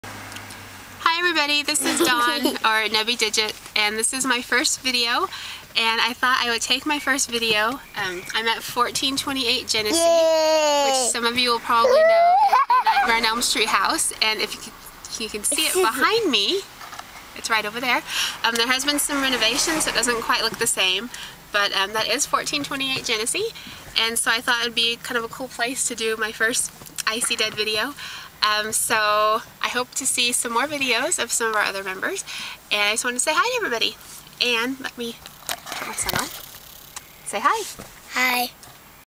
1428 Gennesee Nightmare on Elm Street House
outside of 1428 Gennesee in Hollywood, the house used as Nancy's house for Nightmare on Elm Street.